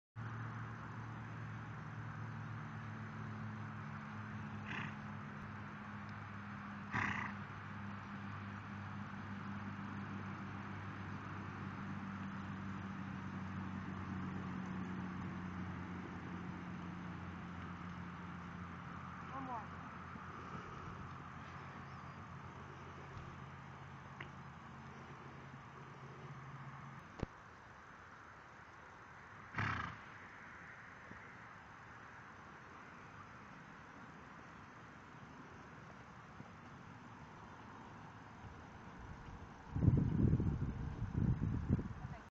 Main Power's My Padre- 3 yr old TWH gelding- barefoot-Jan 8, 2012
Totally barefoot Main Power gelding after dental work and a trying a diffent bit (Myler comfort mullen barrel mouth on short HBT shank)... this was fresh out of the stall, just hopped on and started videoing. He's a cutie. Shown country pleasure and trail ridden extensively. Not for sale.